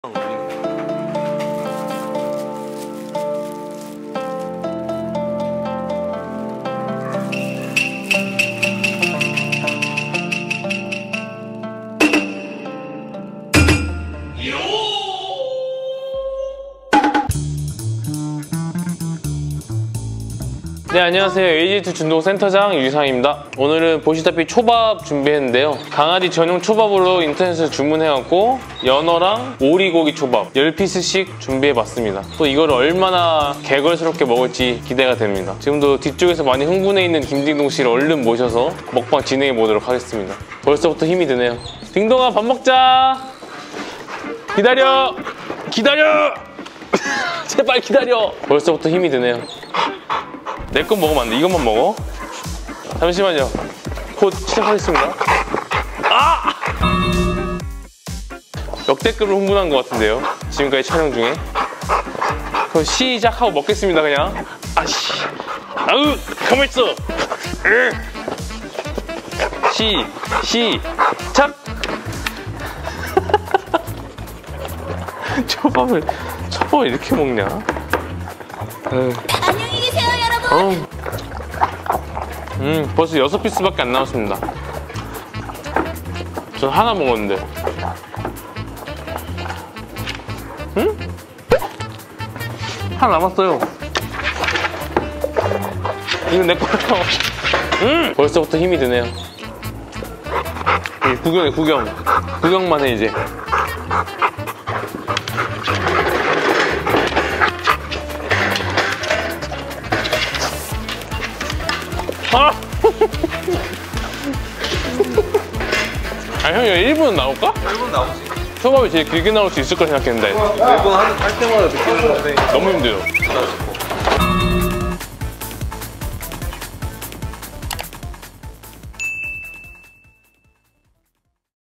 딩동씨 오늘은 일식에 눈을 뜨다!!
연어는 오메가-3 지방산이 풍부한 음식이며,
오메가-3 지방산은 강아지의 면역력 향상에 도움을 주기도 하며,
강아지의 털을 윤기 있게 유지하게끔 하기도 해요!

 또한, 오리고기는 혈중 콜레스테롤 농도를 낮추는데도움을 주고
 비타민 및 철분을 보충해서 모질개선, 피모 개선 등의 효능을 볼 수 있어서

 강아지주식으로도 적당한 고기랍니다~ 하지만!!
어느 음식이나 마찬가지로 적당량을 급여하는 것이 중요합니다!

또한, 모든 음식을 반려견에게 급여하기 전
 소량을 급여하여 알레르기가 있는지 확인해 주세요!!

 딩동이의 허겁지겁 식탐을 부리는 모습이 귀여우셨다면